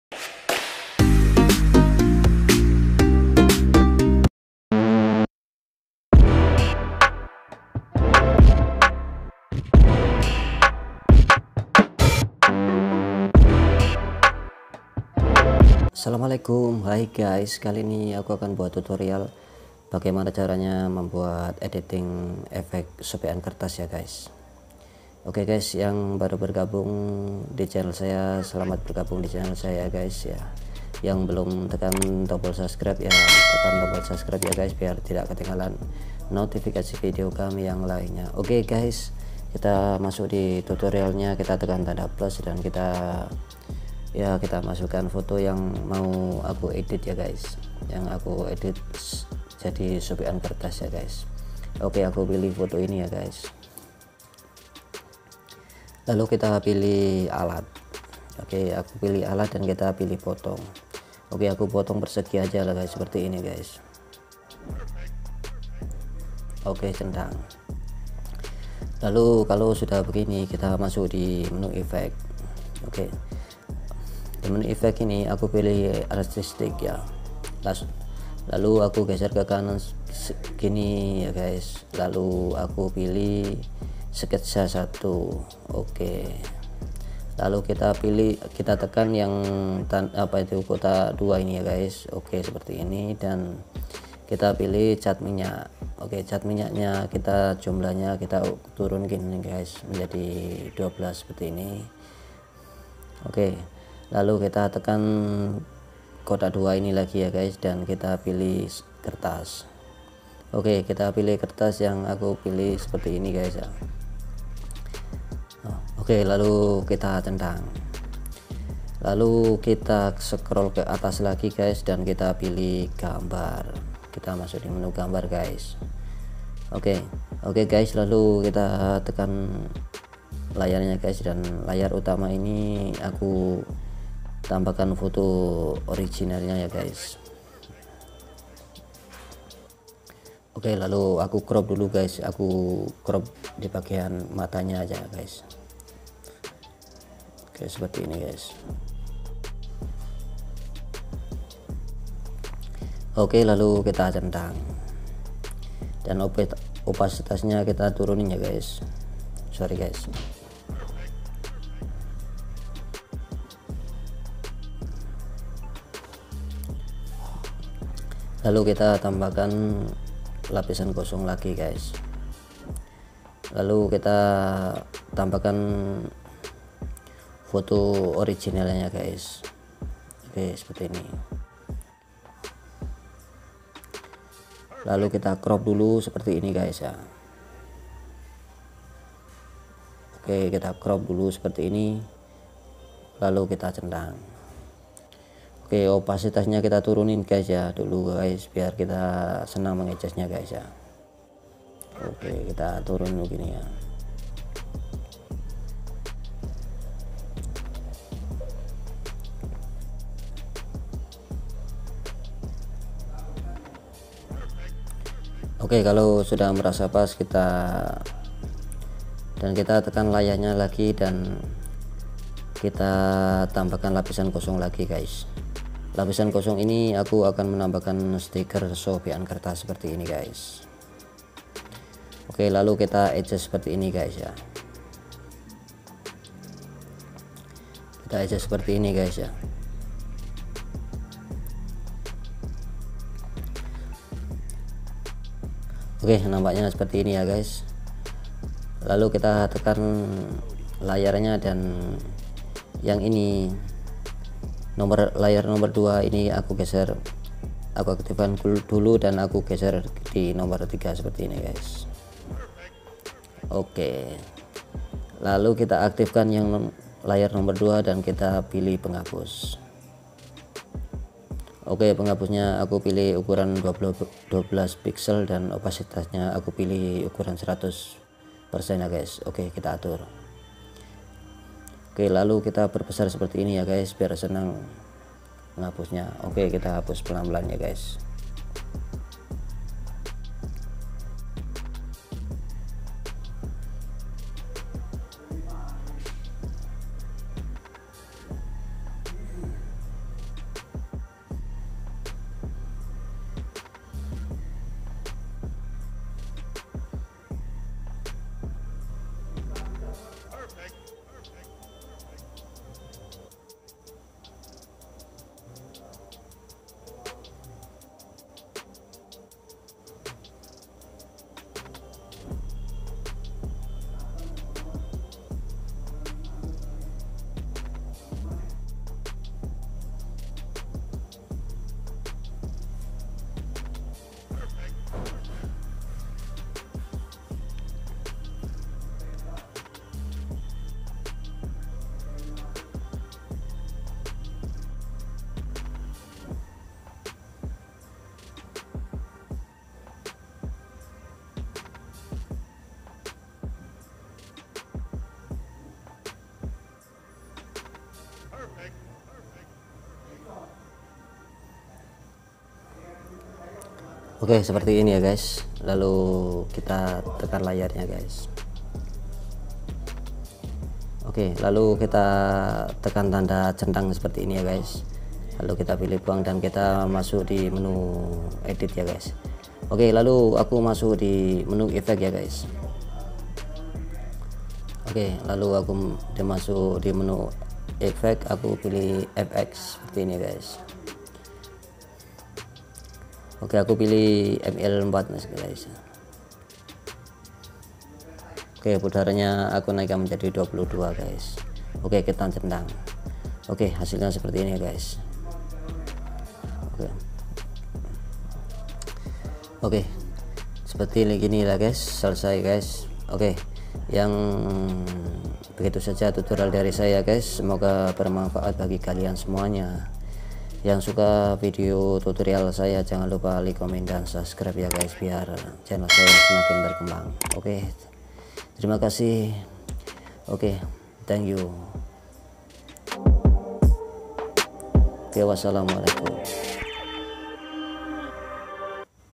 How To Edit Tear Paper Effect Photo on Android | PicsArt Tutorial
Assalamualaikum
in this video I made a tutorial on how to edit the photo of the torn paper effect on android PicsArt

Download PicsArt